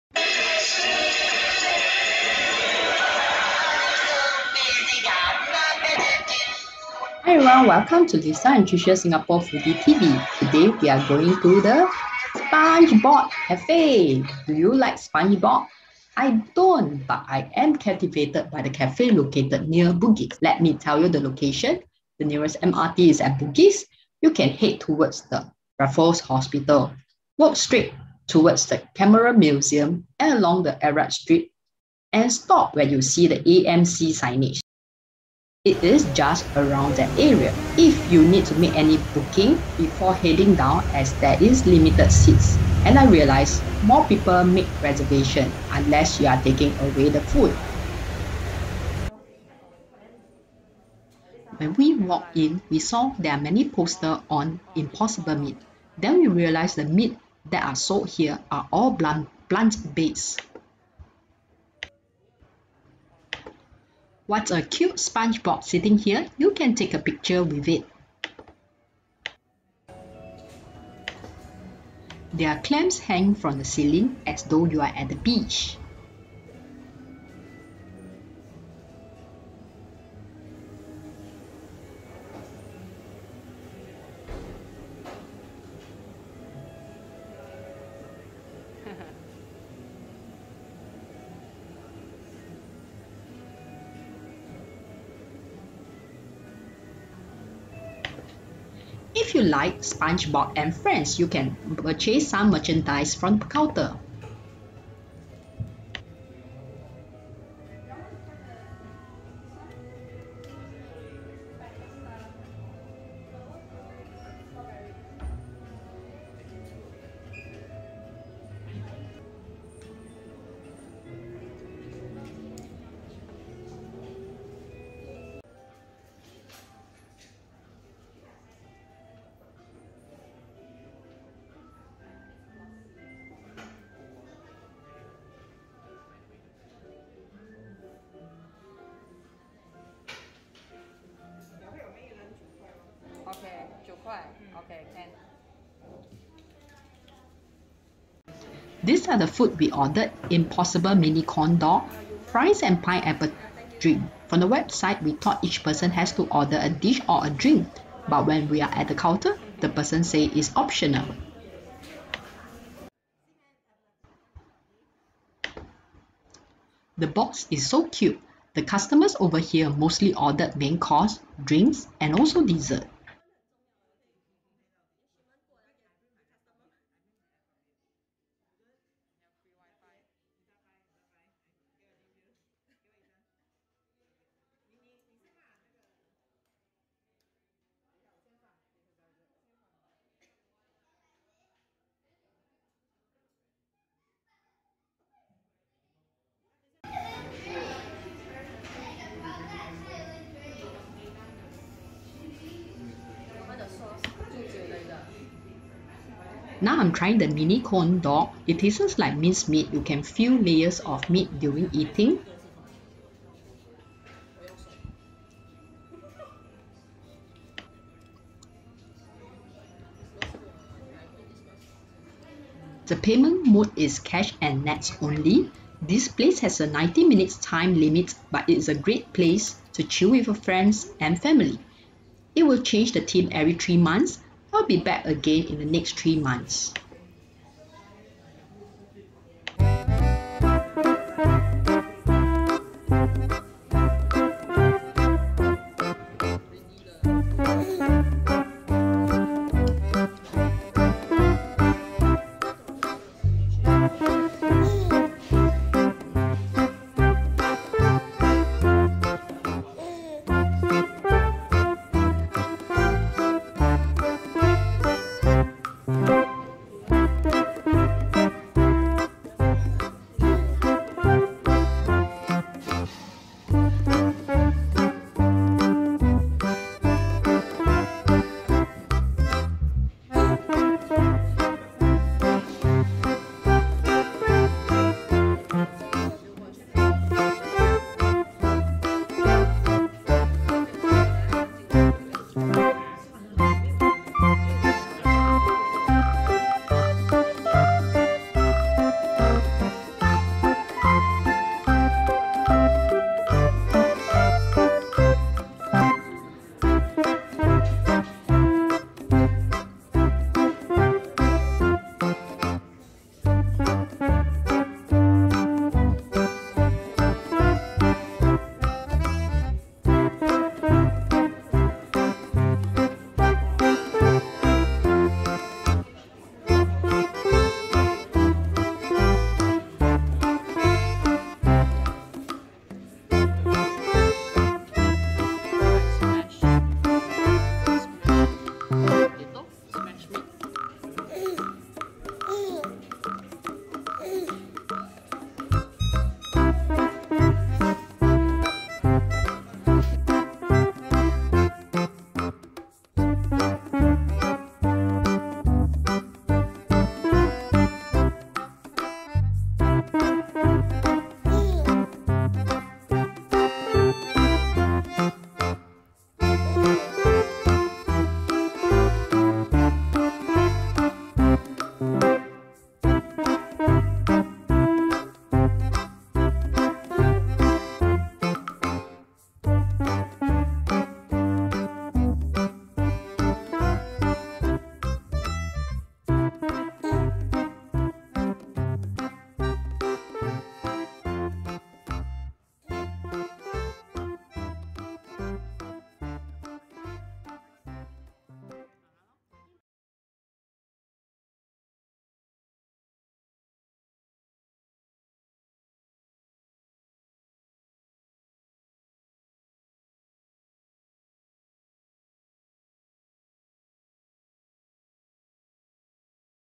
SpongeBob Café in Singapore Must try Bugis
Hi everyone, Welcome to Lisa and Tricia Singapore Foodie TV! Today, we are going to the SpongeBob café! Cartoon… Do you like spongebob? I don’t but I am captivated by the café located near Bugis. Let me tell you the location. The nearest MRT is at Bugis. You can head towards the Raffles Hopsital walk straight to the camera Museum, and along the Arab street and stop where you see the AMC signage. It is just around that area. Let me show with you the you need to make any booking before head down as there is limited seats and I realized more people make reservation unless you are taking away the food.
When we walked in, we saw the many poster on the impossible meat. Then we realized the meat here is all plant based. What a cute spongeBob sitting here, you can take a picture with it. Do you recognize Patrick? There are clams hanging from the ceiling, as though you are at the beach. If you like spongebob and friends, you can purchase some merchandise from the counter. These are the food we ordered, Impossible Mini corn dog, fries and pineapple drink. From the website, we thought each person has to order a dish or a drink. But when we are at the counter, the person said is optional. The box is so cute. The customers over here mostly ordered main course, drinks and dessert. Now, I’m trying the mini corn dog. It tastes like minced meat, you can feel layers of meat. The payment mode is cash and nets only. This place has a 90 mins time limit  but it is a great place to chill with your family and friends! It will change the theme every 3 months. I will be back again in next 3 months time.

Recommended videos for SpongeBob café